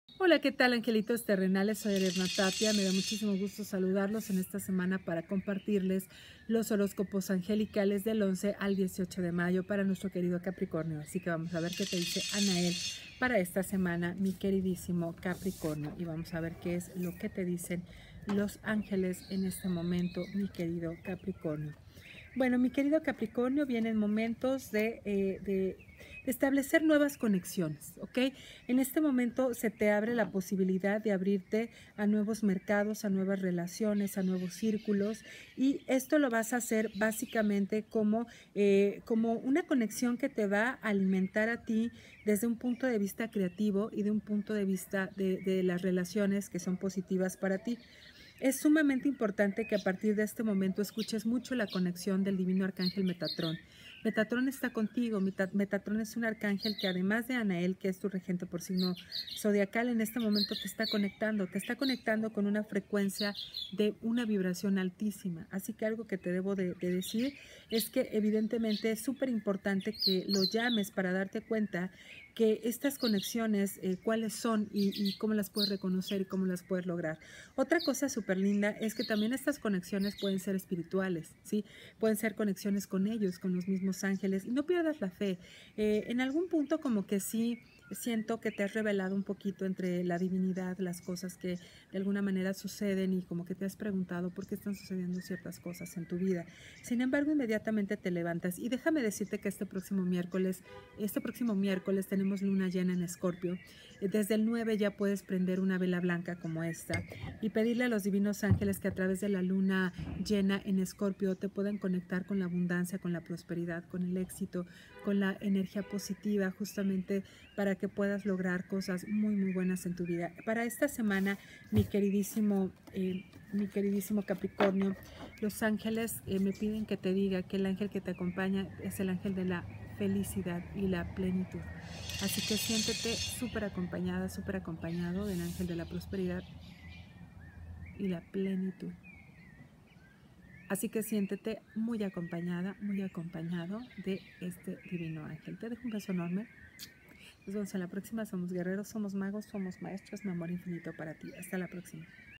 Ariadna Tapia Capricornio 11 al 18 de mayo de 2017.
El 9 y 10 de mayo tenemos luna llena en Escorpión. Recomiendo encender una vela blanca esos días y pedir a los ángeles abran el camino y eliminen negatividades.
Ariadna Tapia, angelóloga y life coach te comparte los horóscopos angelicales para la semana del 11 al 18 de mayo de 2017.
Facebook Ariadna Tapia Angelologa Twitter AriadnaTapiaOk